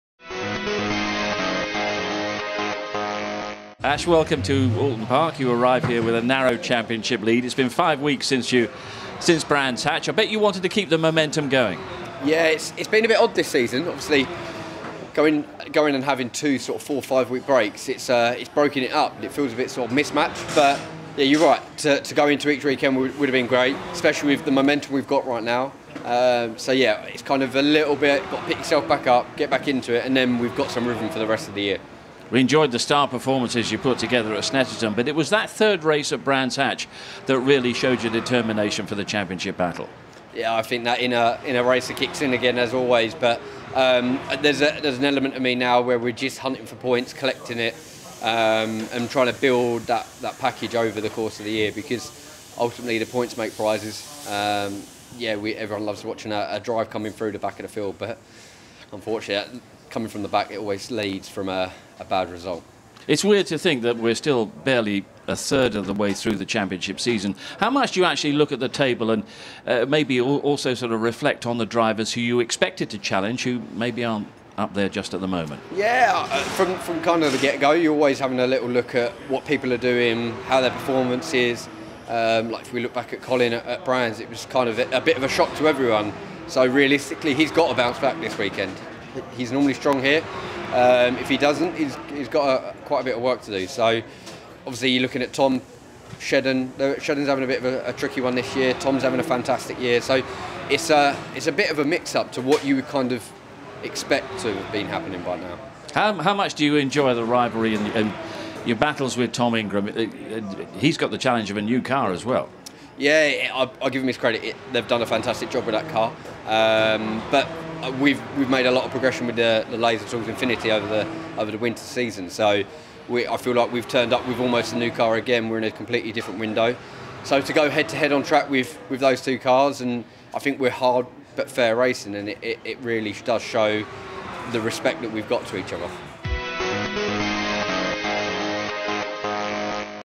Steve Rider catches up with Ash Sutton | BTCC 2021
Steve Rider caught up with Ash Sutton on Saturday morning to discuss how the Championship is shaping up for him.